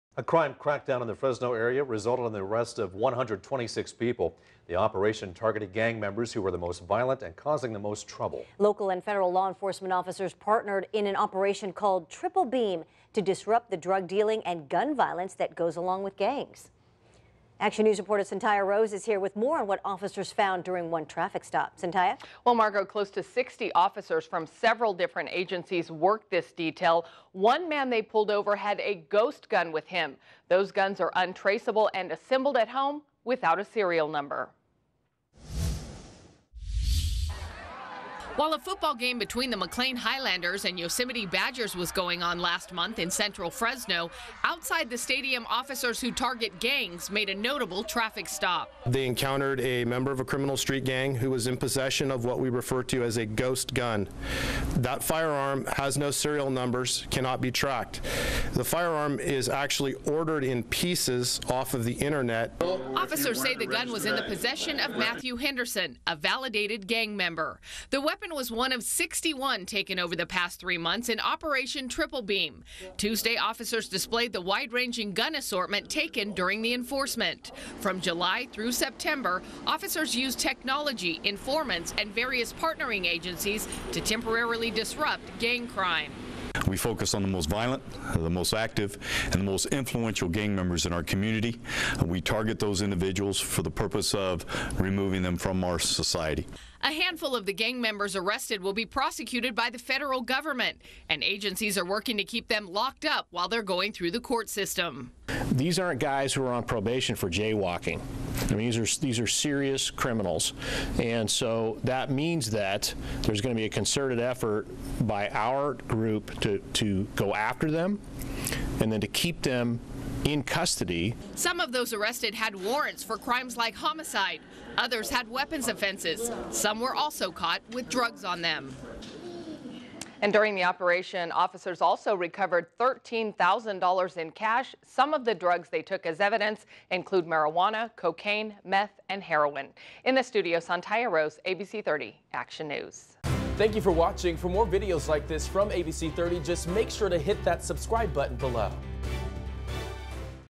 Fresno area gang bust: 126 arrested, 61 guns seized during multi-agency operation
From July through September, officers used technology, informants and various partnering agencies to temporarily disrupt gang crime in Operation Triple Beam.

The crime crackdown resulted in the arrests of 126 people and seizure of 61 guns in the Fresno area.